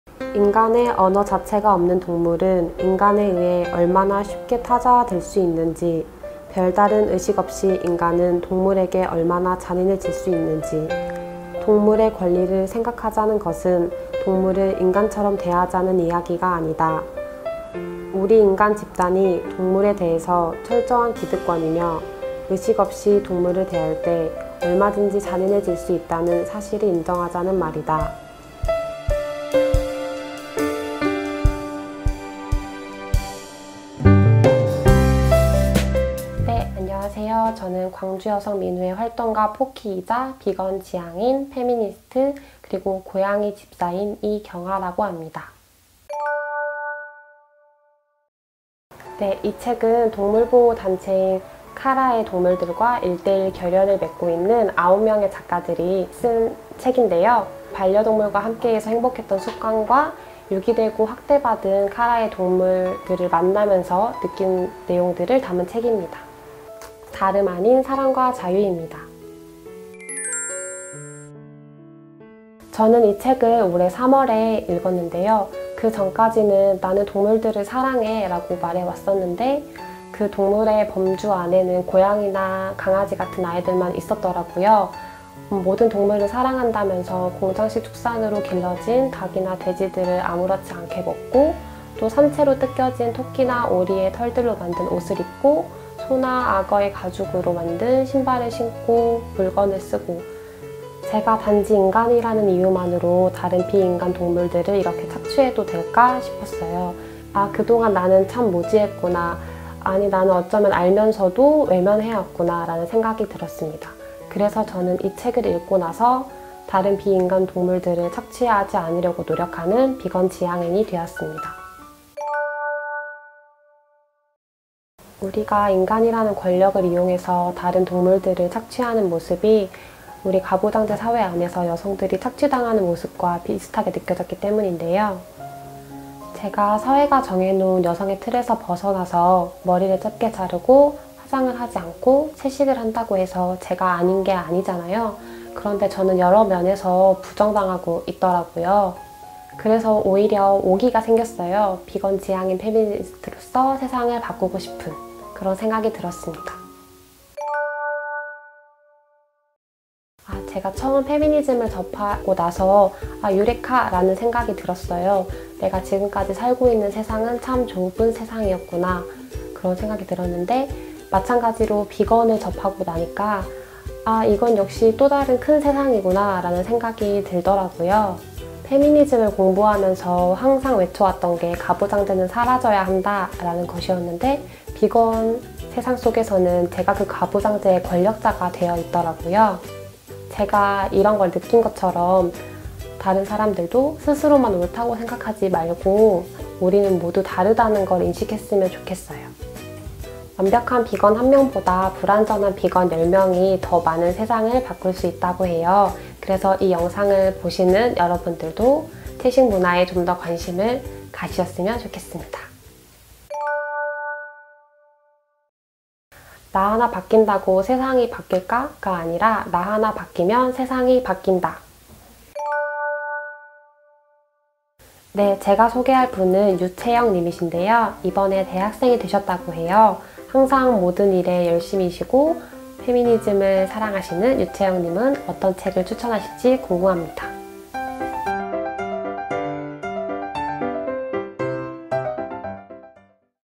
[내인생의 나비책] 열한번째 도전자 이경아
"스무 권의 책, 스무 개의 성평등 이야기"
광주여성가족재단 북카페 개관기념
내인생의 나비책 릴레이
스무 색깔의 광주 여성 이야기가 펼쳐집니다.

댓글을 달아주신 분들 중 추첨을 통해 나비책과 은새암 노트를 드립니다.